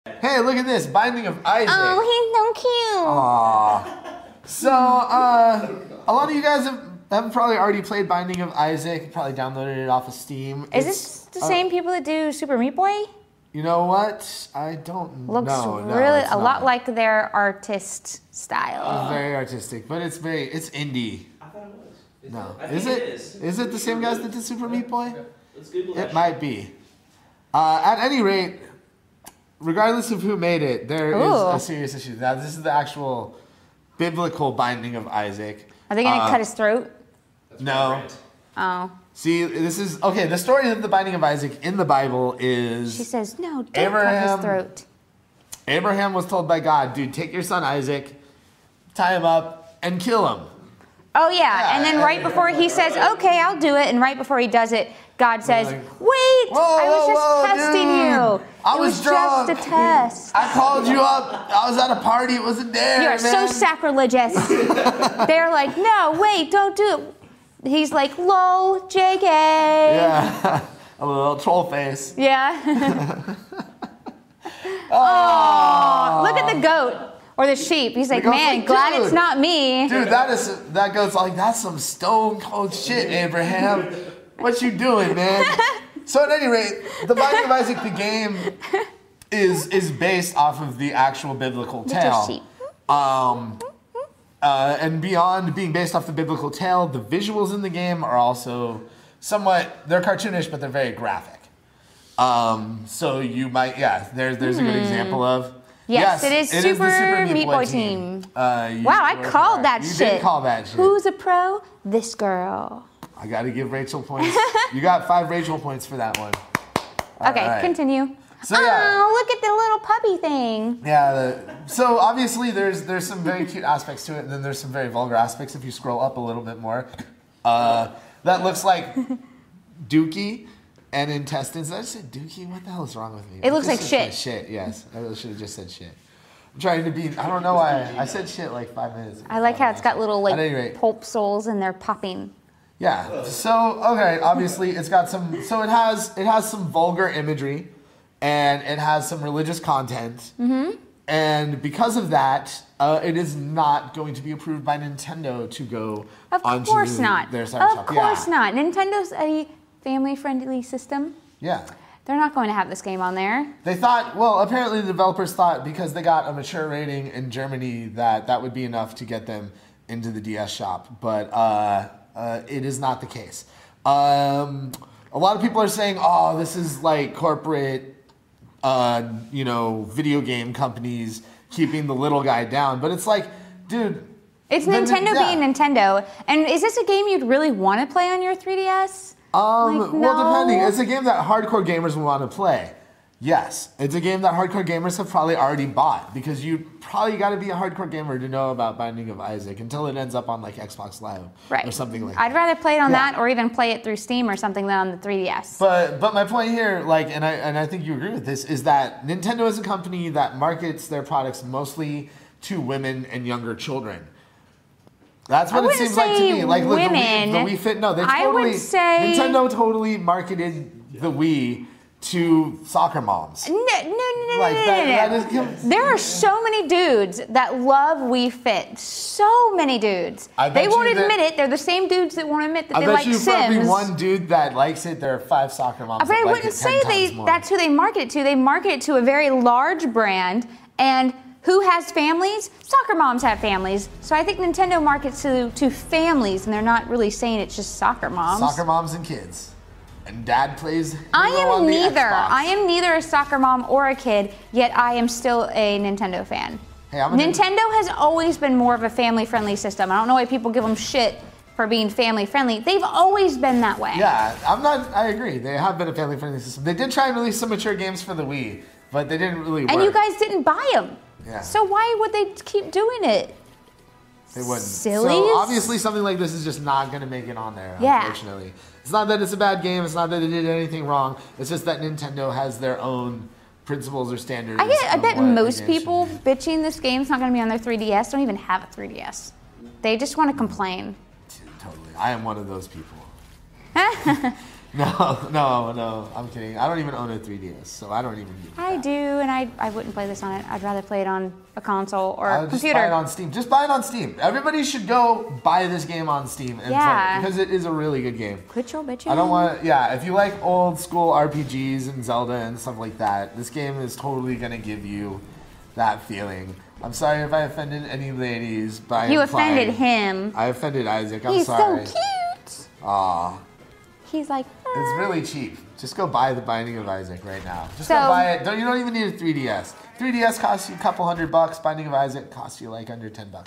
Blake and Raychul discuss The Binding of Isaac NOT on 3DS - Week In Geek Ep. 5
Blake and Raychul talk about The Binding of Isaac, why Nintendo doesn't want it near the 3DS, intestines, poop, and the Bible. Watch full episodes and tune in live every Friday at Noon Pacific -- Recorded live on Justin.tv